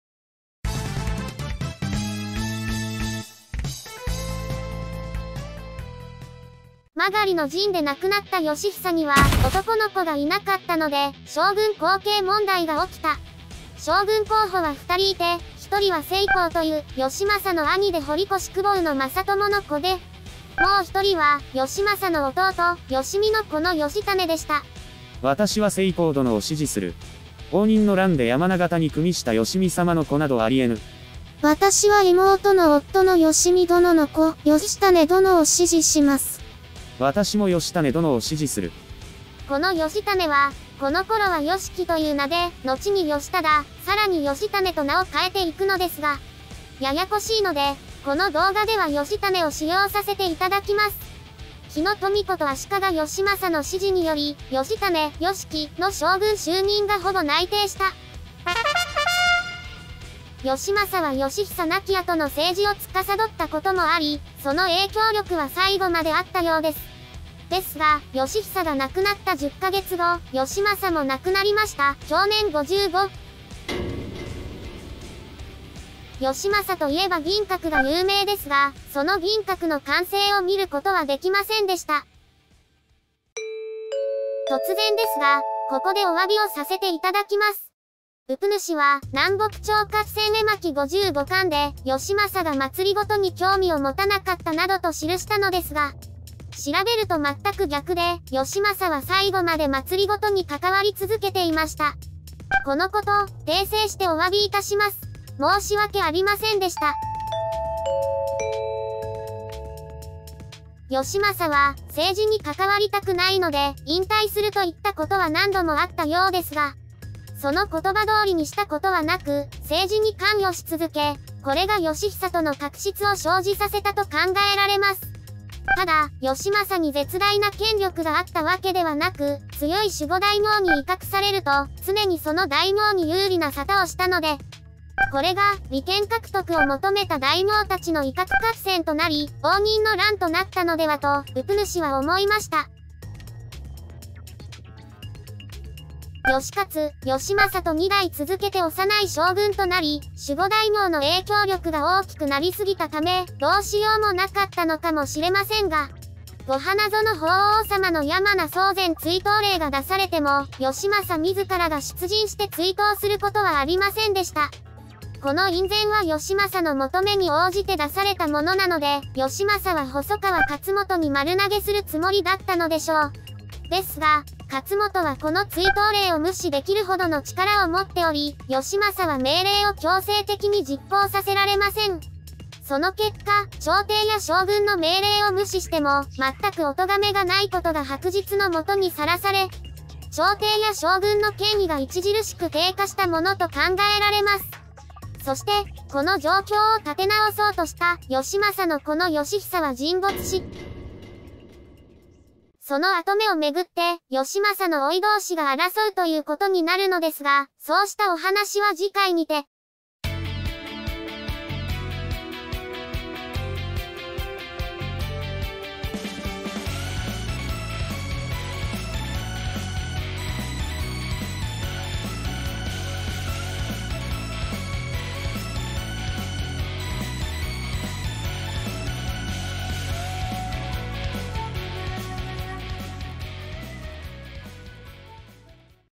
戦国合戦絵巻、第21巻[足利義政の晩年]
足利義政の晩年をアニメにしました